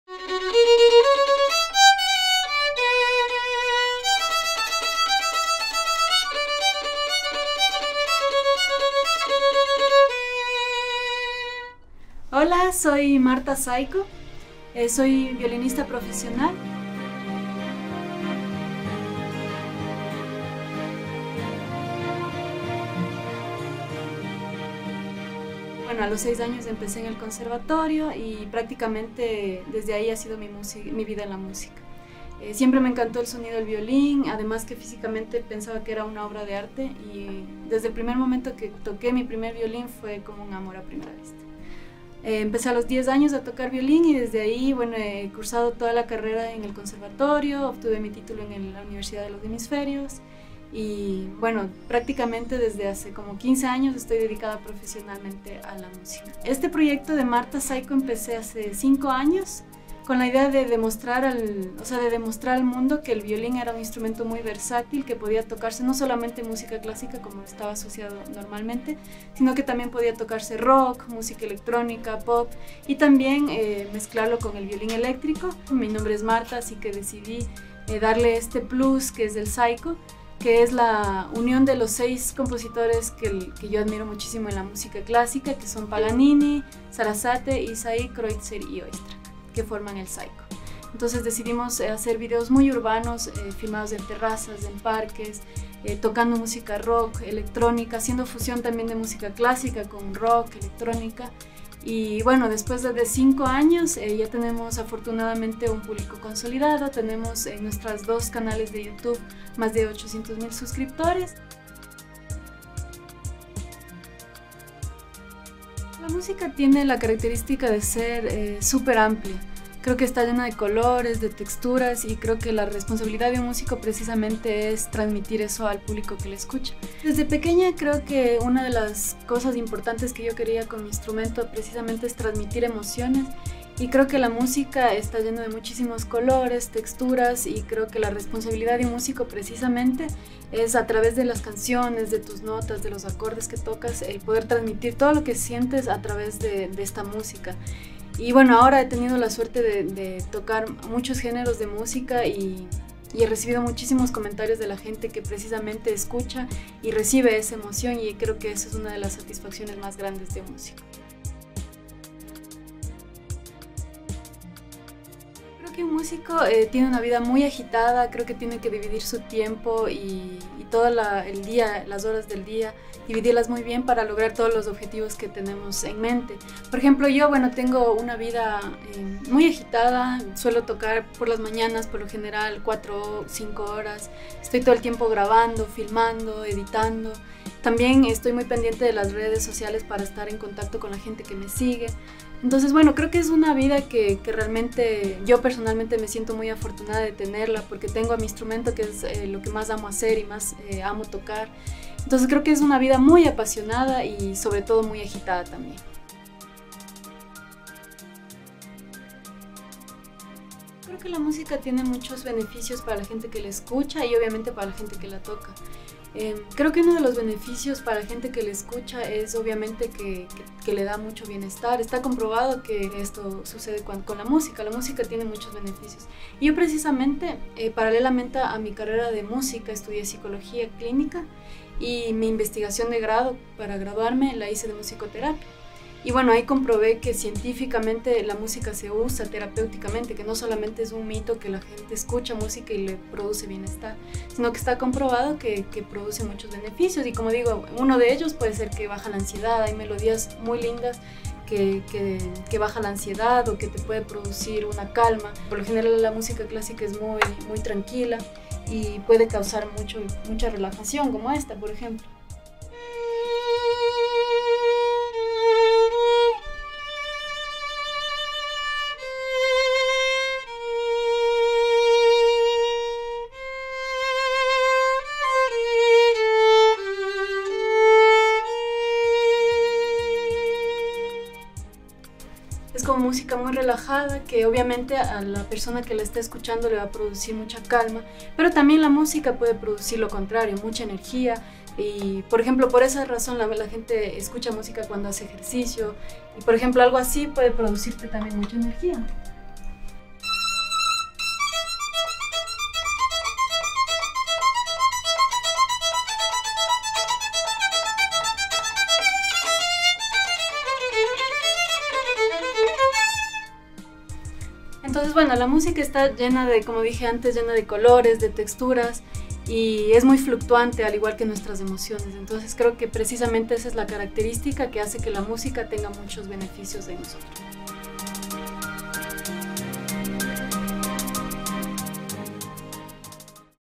Episodio 3 - Martha Psyko (Parte 1)
Para @marthapsyko, el violín fue amor a primera vista. Toca desde los 10 años y ha podido encontrar su propio estilo, lo que la ha llevado a tener más de 800.000 suscriptores en sus canales de Youtube. Conócela y disfruta de su música en la primera parte de esta entrevista